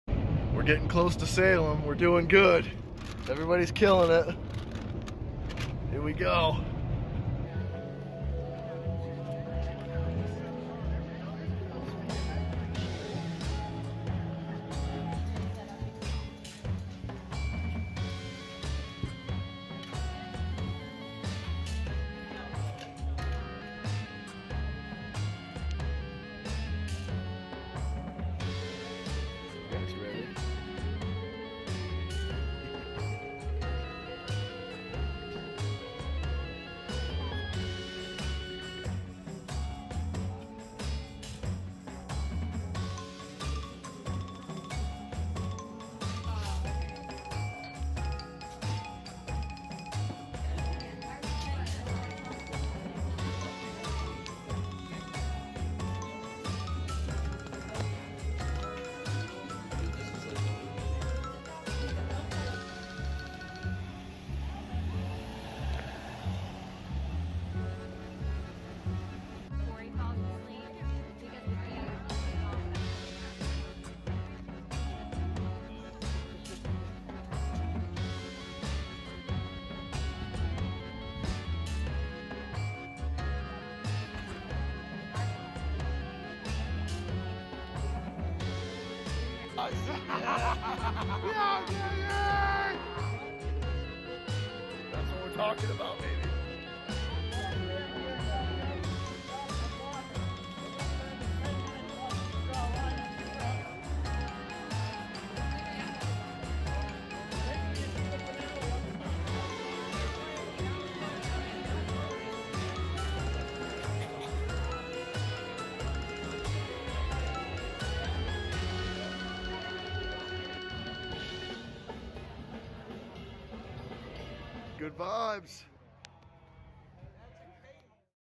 WITCH CITY / A BRIEF VIDEO OF OUR TRIP TO SALEM, MASSACHUSETTS
We recently took a trip to Salem to indulge in the festivities for a night! If you're a fan of horror and history, Witch City is the place for you! They have awesome attractions and plenty of tours to check out! I hope you like the video.